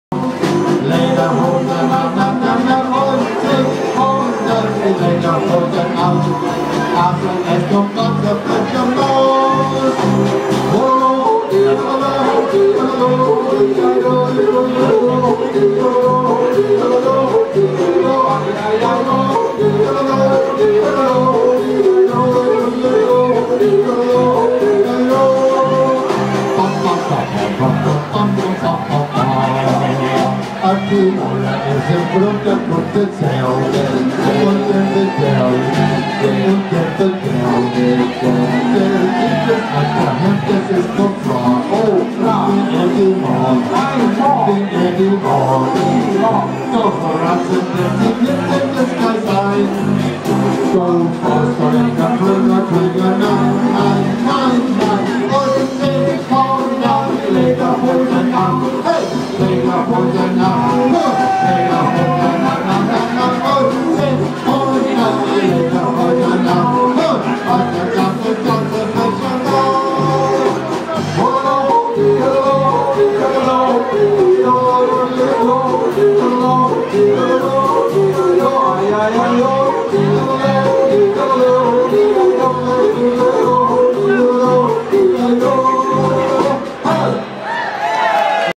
Singing and dancing at Oktoberfest Zinzinnati 2013
A small group dances and enjoys the entertainment during Day 2 of Oktoberfest in Cincinnati. I have no idea what anyone is saying and hope its nothing that offends.